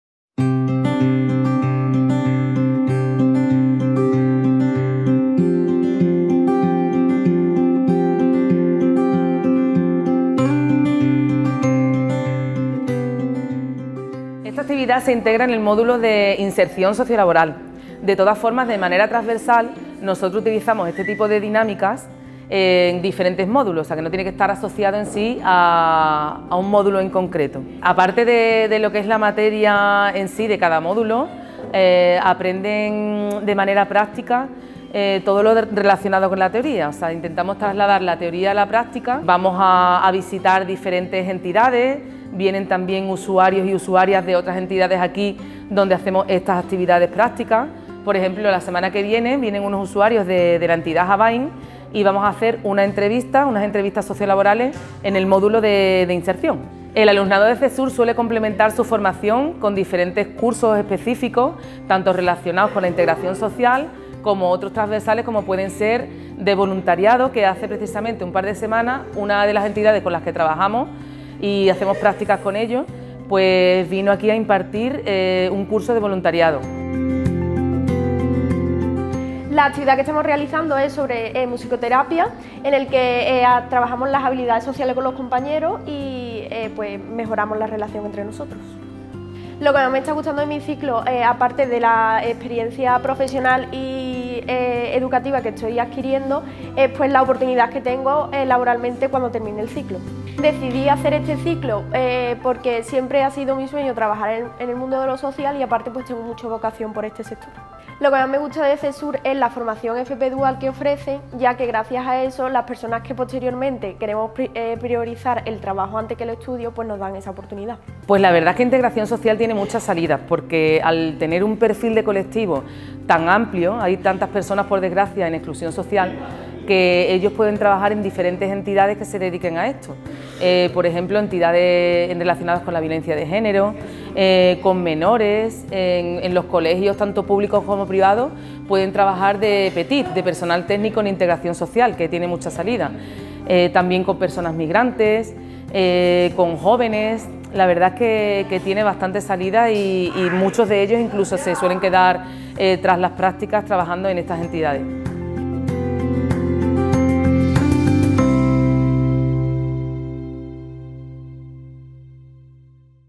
Inserción laboral | FP Dual Integración Social 🧡💚
Hablamos con nuestras alumnas de la FP Superior de Integración Social de Cesur Sevilla, mientras están en el módulo de Inserción Sociolaboral.

En este módulo visitan entidades, reciben charlas y realizan talleres como el de musicoterapia

¿Quieres unirte a la FP de Cesur?
🚨 Llámanos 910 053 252 y matricúlate 🚨